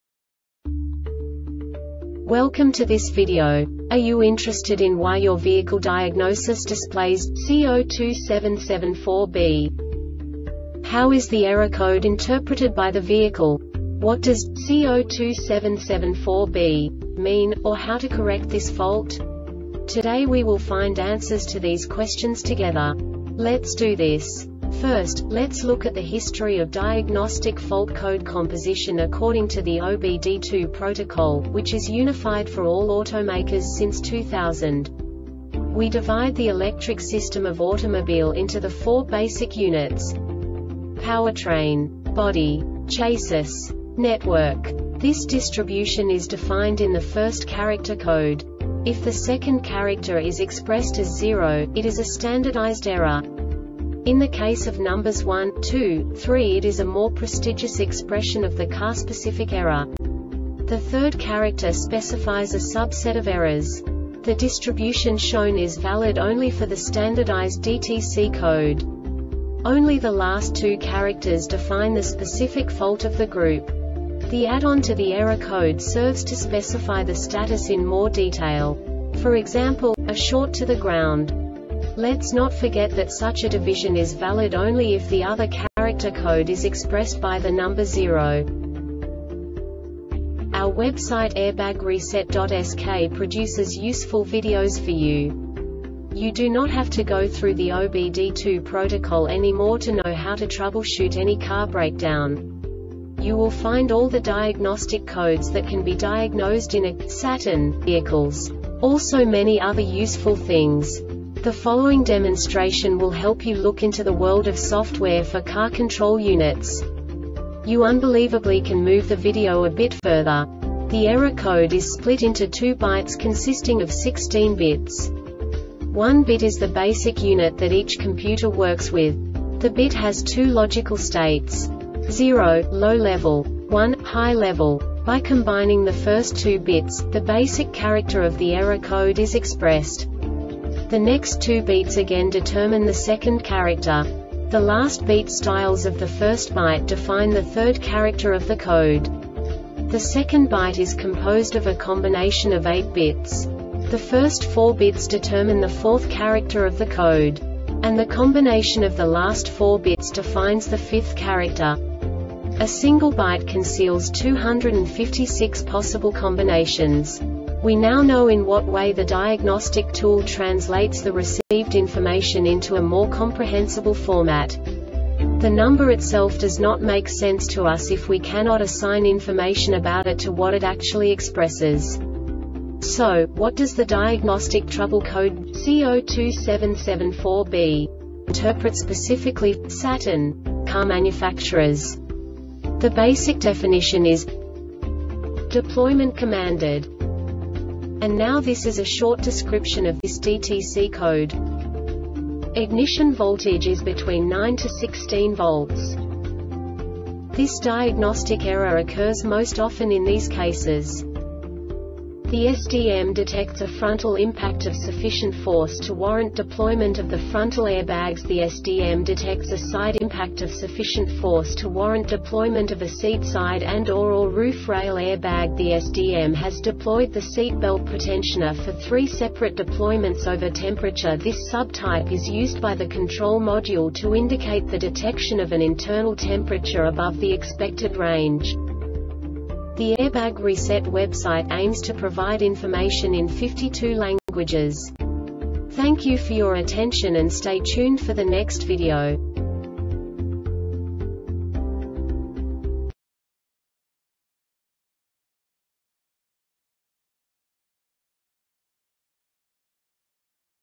DTC Saturn C0277-4B Short Explanation
Contents:
0:21 Basic DTC analysis according to OBD2 protocol standard.
1:48 Insight into programming
2:58 A diagnostically understandable description of the Diagnostic Trouble Code
3:05 Basic error definition

Make:
Saturn
Code:
C0277 4B
Definition:
Deployment Commanded
Description:
Ignition voltage is between 9-16 volts.
Cause:
The SDM detects a frontal impact of sufficient force to warrant deployment of the frontal air bags.The SDM detects a side impact of sufficient force to warrant deployment of a seat side and or or roof rail air bag.The SDM has deployed the seat belt pretensioner for 3 separate deployments.
Failure Type:
Over Temperature